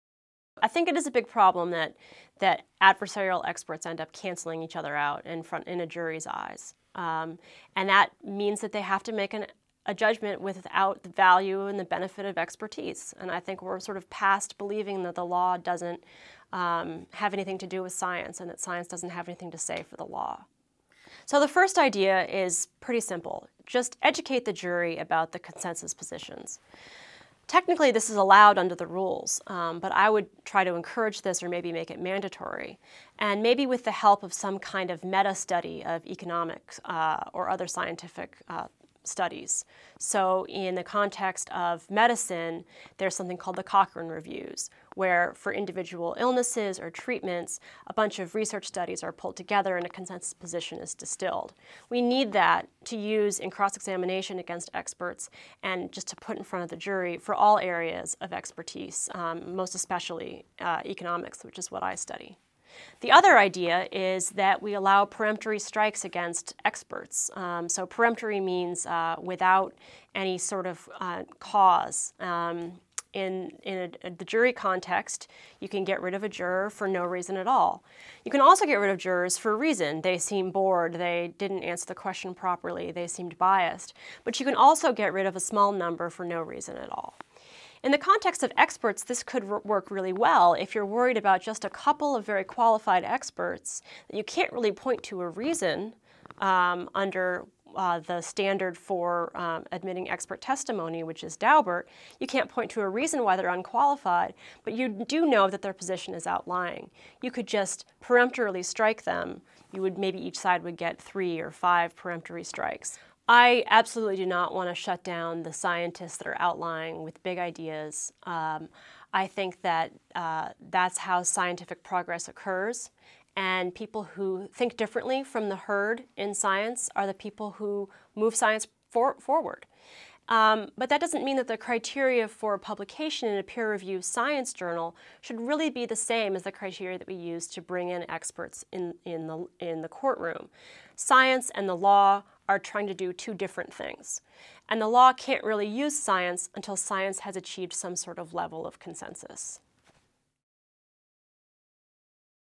Vanderbilt University Professor Rebecca Haw on Dueling Expert Witnesses
Dueling expert witnesses can give inaccurate view of science. Vanderbilt legal scholar Rebecca Haw suggests preemptive strikes against experts to prevent outliers from giving an unsubstantiated perspective of the facts to jurors.

Even if 90 out of 100 experts believes the same scientific principle, if there are a somewhat balanced number of experts testifying on each side, to jurors, it appears that scientific consensus on the subject is divided roughly 50-50.

            "It's called the 99-1 problem," says Rebecca Haw, assistant professor of law at Vanderbilt Law School. So, even if there's 99 experts who will say the world is round, if the other side can find just one to say the opposite, the real scientific consensus on the issue might not get through to jurors.

            In new research, Haw gives two suggestions on how to address this issue. First, encourage or require testimony on the status of consensus within the scientific community. Second, Haw believes a "preemptive strike" system -- the same as used for jurors -- might be useful for expert witnesses.

"The experts that are the most damaging to any one side wouldn't be used," Haw said. "Sometimes those would be the best experts," she admits. "But if you can call any of 99 experts who will say virtually the same thing, then you just go down the list," she said. "But if you have only one expert in the first place, you might not bring the case to court if you knew that a preemptive strike might wipe out your best witness."

Scientific consensus tends to shift over time, so there would be cases under Haw's system where an "outlier" expert who is actually right might find themselves forbidden to testify.

"Scientific consensus, while wrong sometimes, is the best thing we've got," Haw said. "We need to be able to organize our affairs around what's generally accepted to be true at any time, rather than try to hit a moving target of where we think science is going."